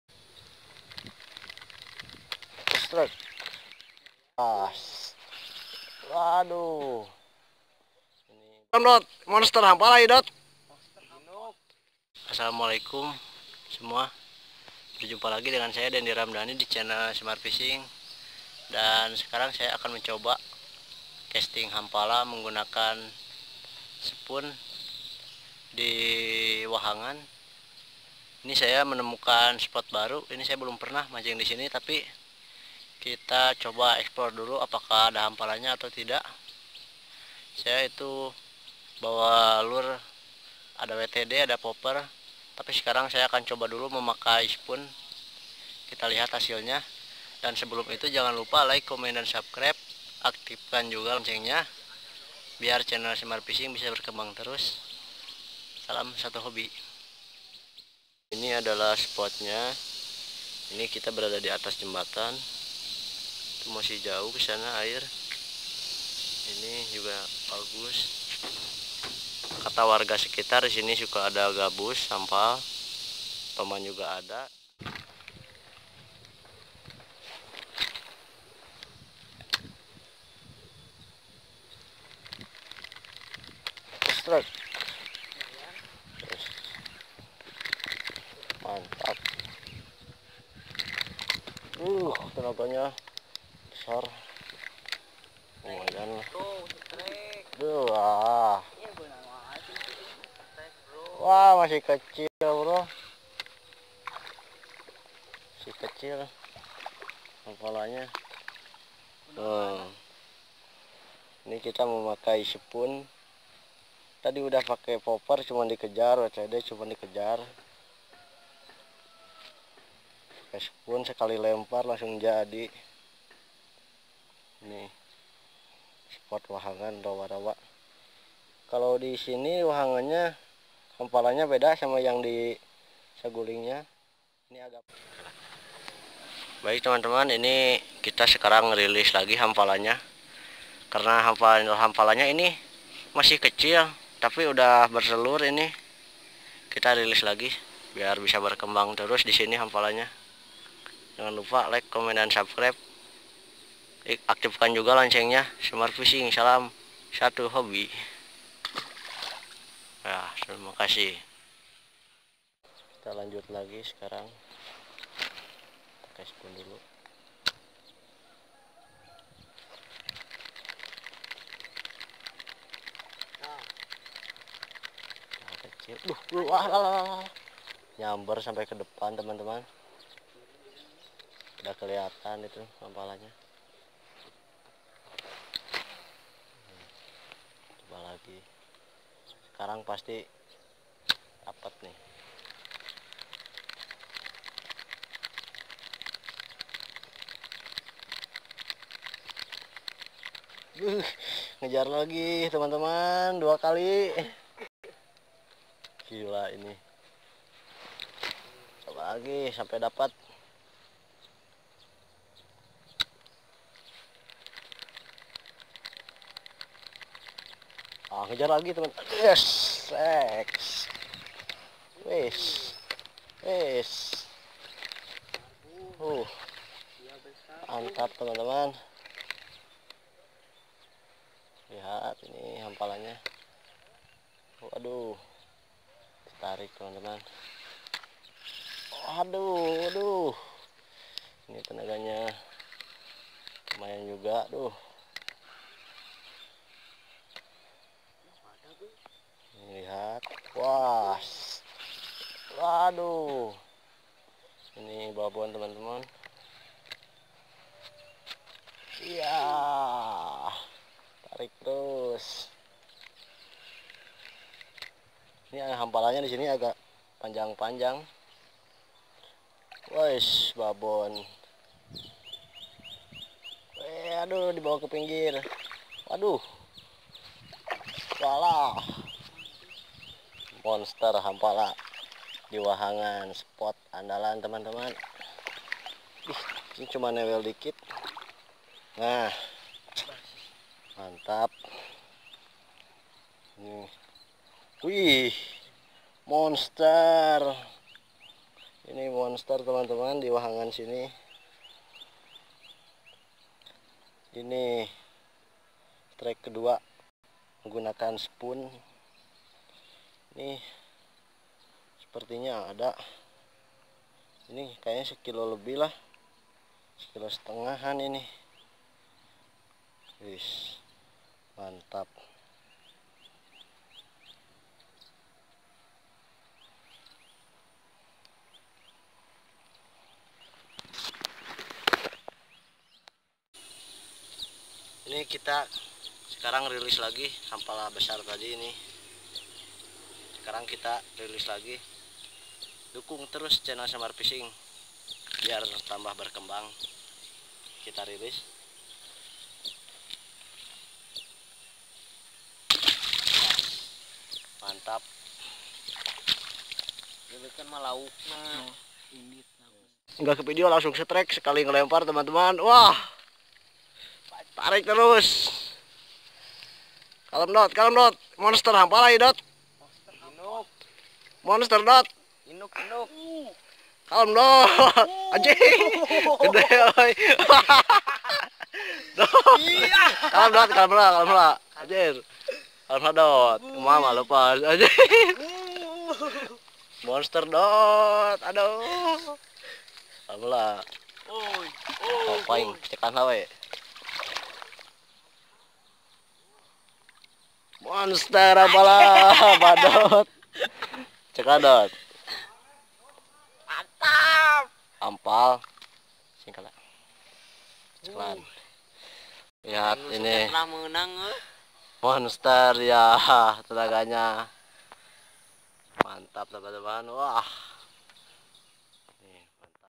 SUNGAI KECIL, Banyak Ikan Hampala | Casting Sebarau Sungai
Kali ini saya menemukan sebuah sungai kecil dan saya curiga ada ikan hampala sebarau nya disana.
Setelah saya coba inilah hasilnya.
Memuaskan sekali 🙏🙏🙏
Saya casting di spot ini baru pertama kali.

mancing hampala di sungai, casting hampala arus deras, casting sebarau sungai, casting hampala sungai, casting sebarau arus deras, sungai kecil banyak hampala.

Salam NYANGUT!